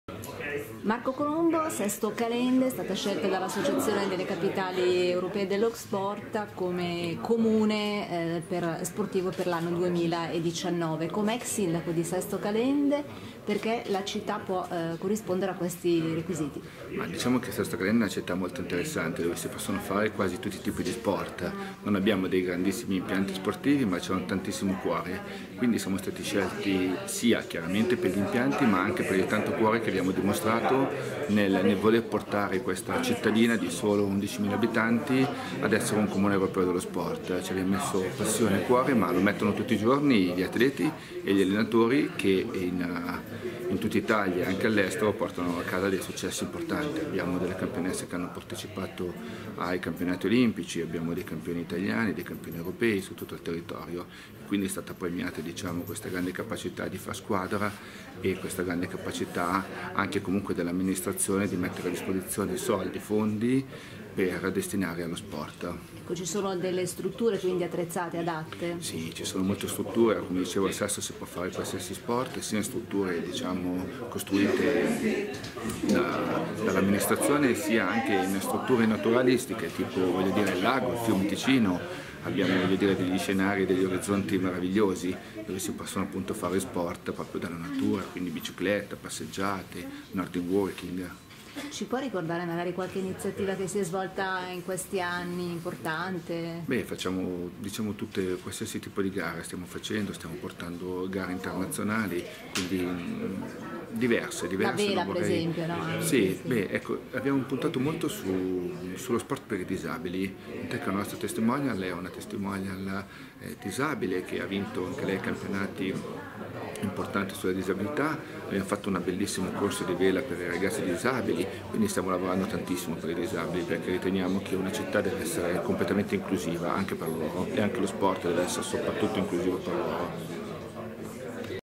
Colombo: "Sesto Calende Comune Europeo dello Sport"
Milano, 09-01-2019 Passione e cuore, oltre che strutture e la capacità di fare squadra e di essere inclusivo: questi i fattori che hanno permesso a Sesto Calende (Va) di essere candidato a 'Comune Europeo dello Sport 2019', come ha sottolineato l'ex sindaco e attuale consigliere regionale Marco Colombo, intervenuto alla presentazione del progetto a Palazzo Lombardia. La candidatura rientra nel riconoscimento che ogni anno 'ACES Europe', assegna ai Comuni con meno di 25.000 abitanti che si sono distinti per progetti sportivi dal valore anche etico ed educativo.